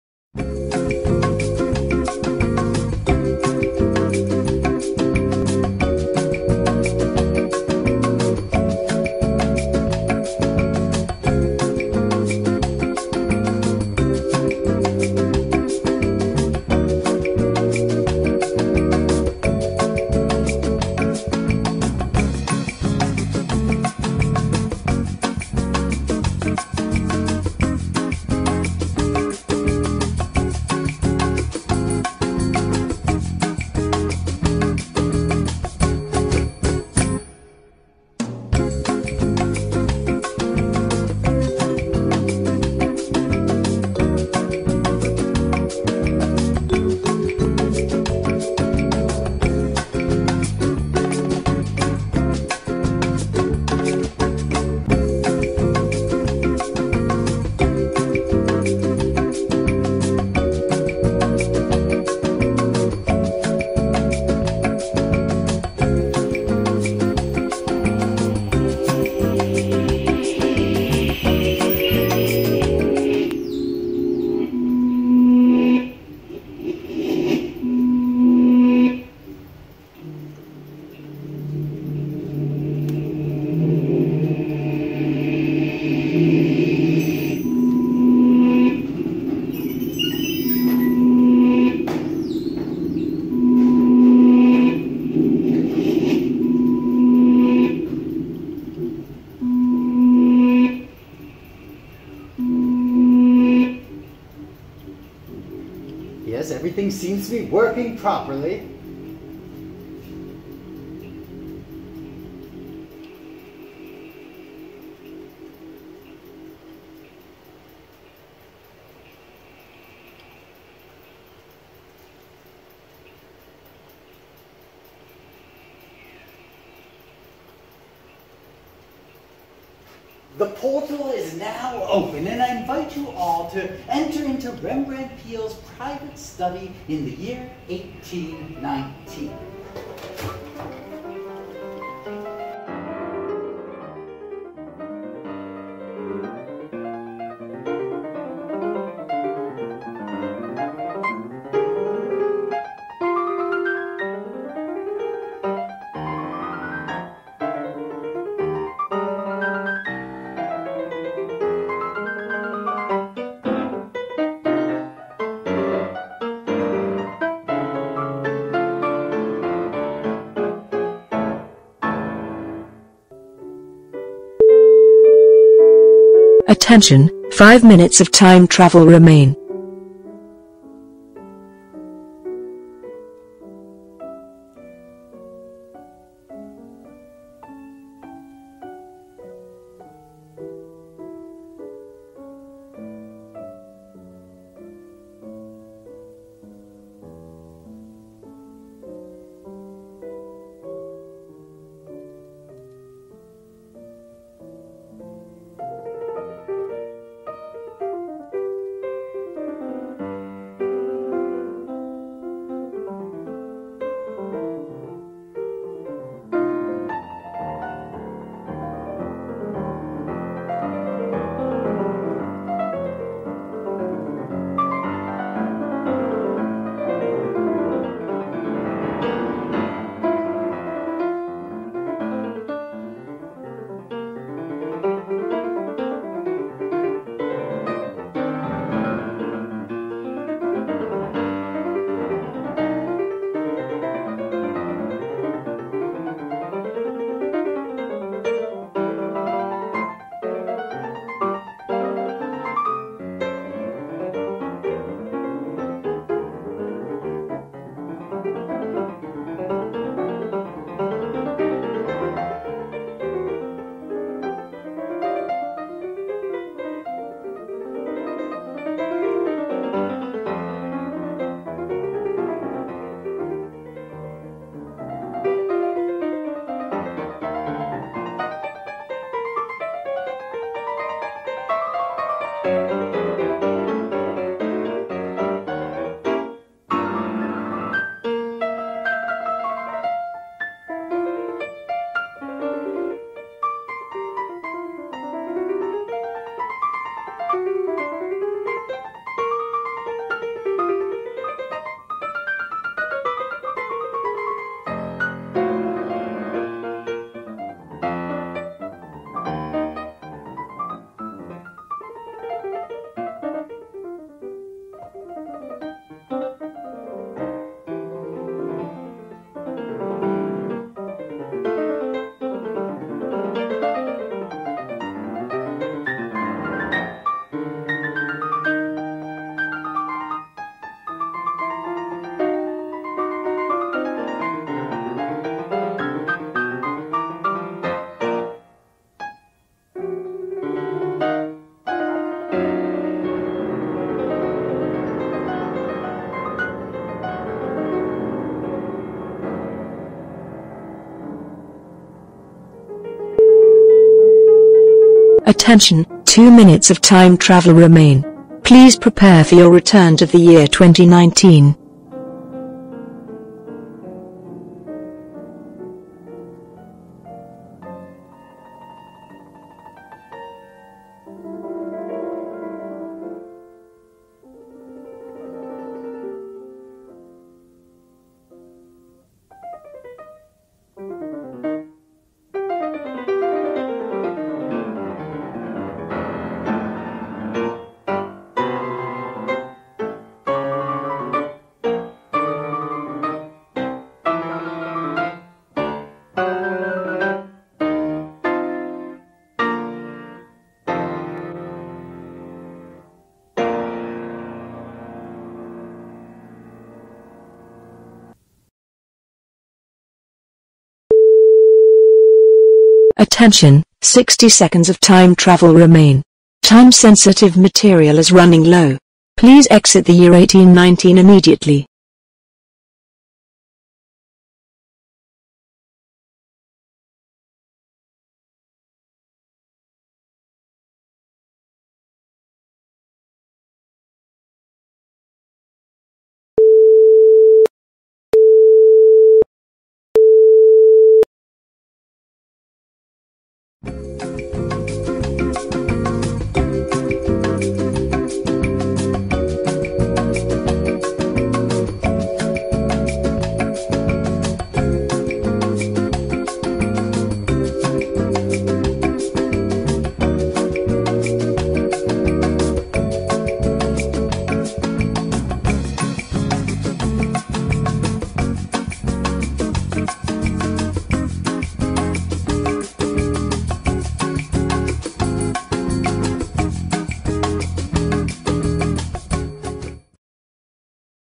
Time Travel Tours at the Peale - 8 Minute Recap
From November 2018 – August 2019, the Peale hosted a bureau of the Escapement Time Travel Agency at the Peale Museum in Baltimore, MD, inside the oldest museum building in America.

After activating our 200-Year Time Machine, visitors embarked on a guided tour of Rembrandt Peale’s study in the year 1819.

ALL TIME TRAVEL TOURS INCLUDED UP TO  40 MINUTES OF ACTUAL TIME TRAVEL!